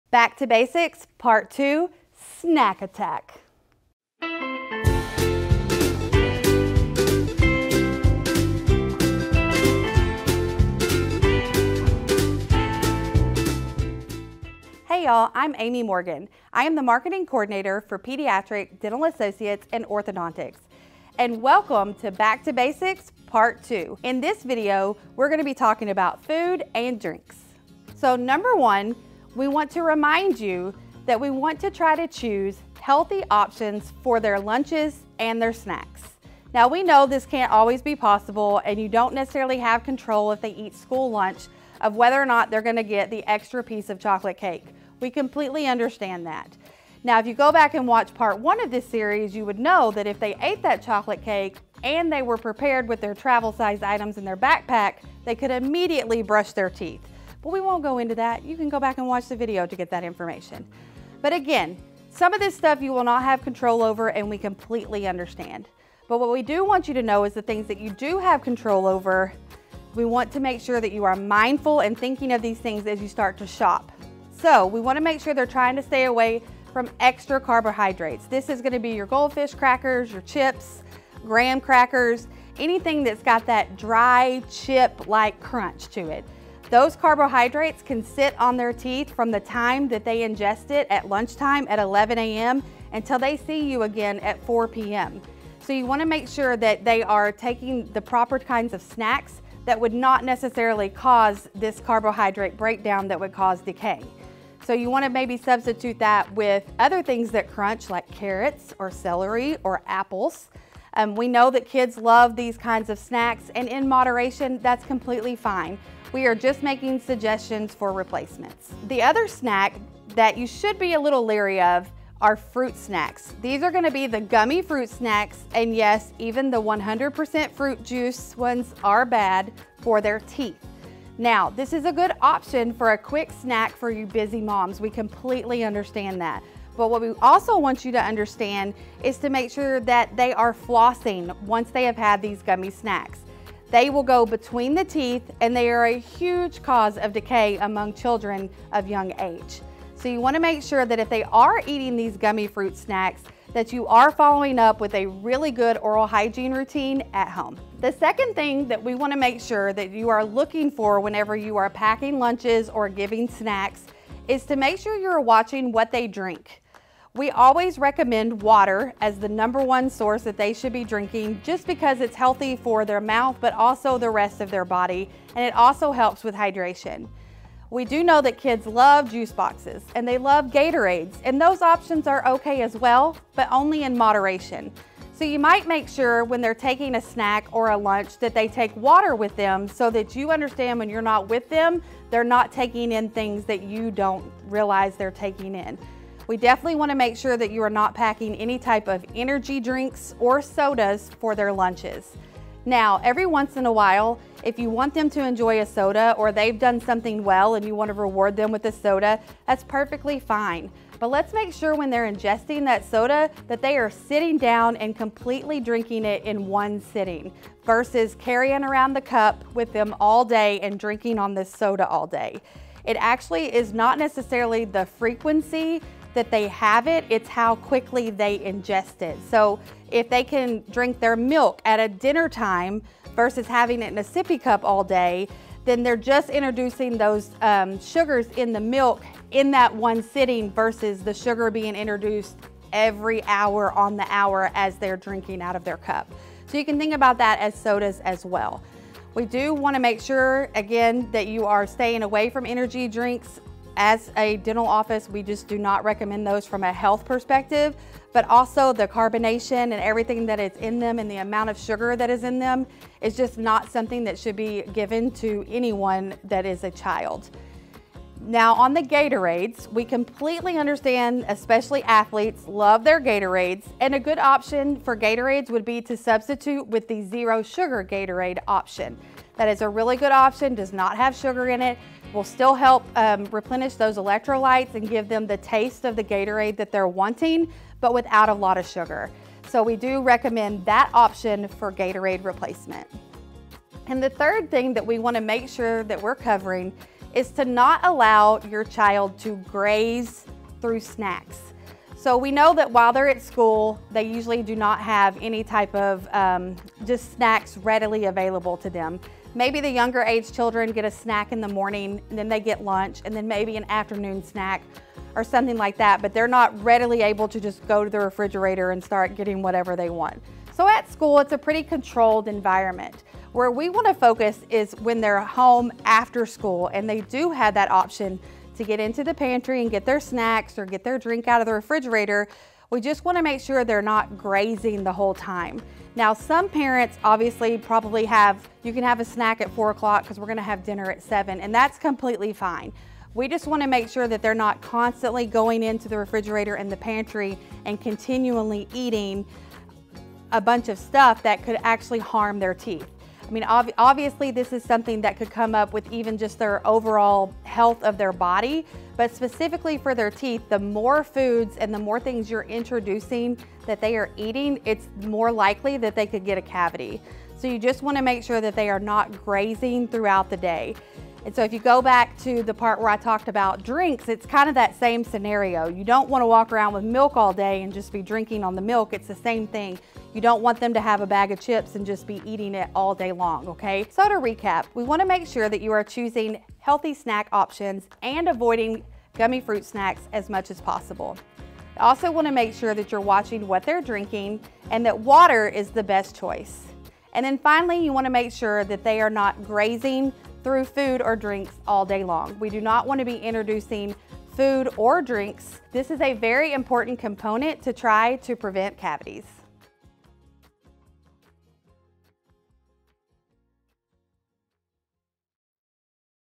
Back to Basics: Snack Attack
With school starting back up this month, we are taking a few weeks to get back to the basics and help you get back into your normal routine so that you and your children can thrive. This week we’re focusing on which school lunches, snacks, and drinks are best for your child’s smile!